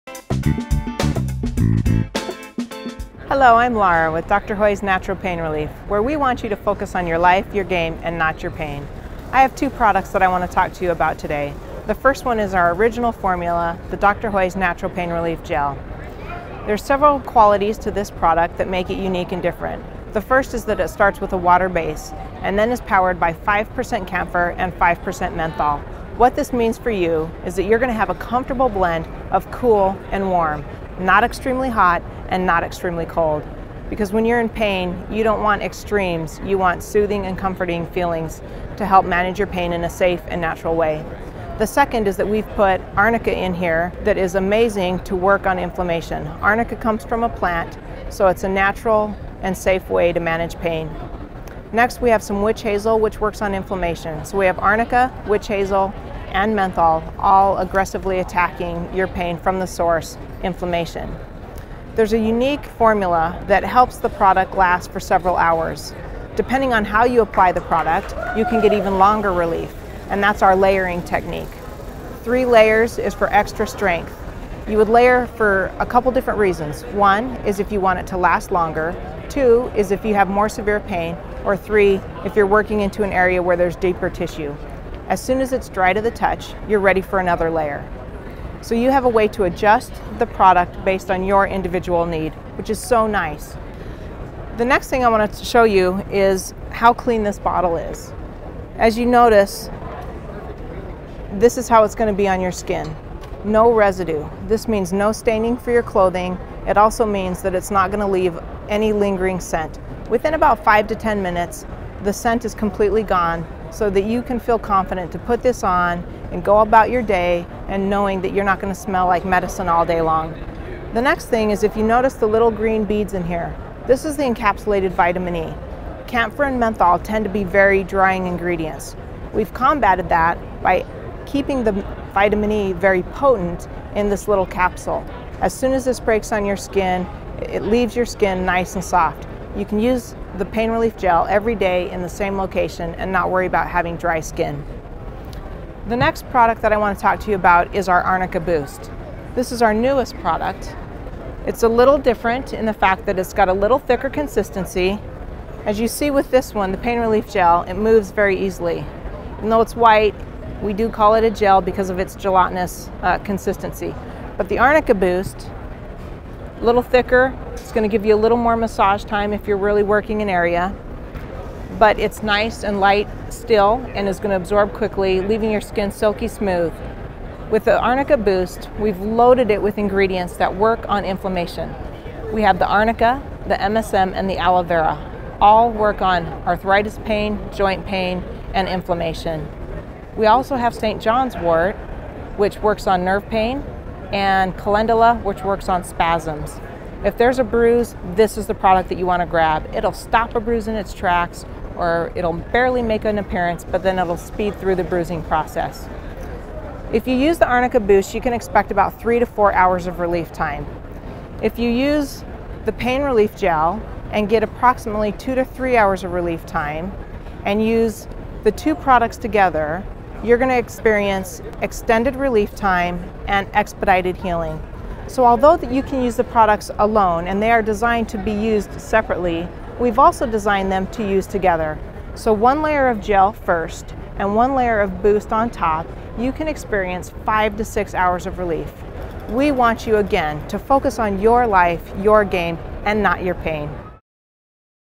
Dr. Hoy's Pain Relief Gel and Arnica Boost
Laura From Dr. Hoy's talks about the Pain Relief Gel, and the Arnica Boost.

Be sure to visit TriSports.com to pick up these great products!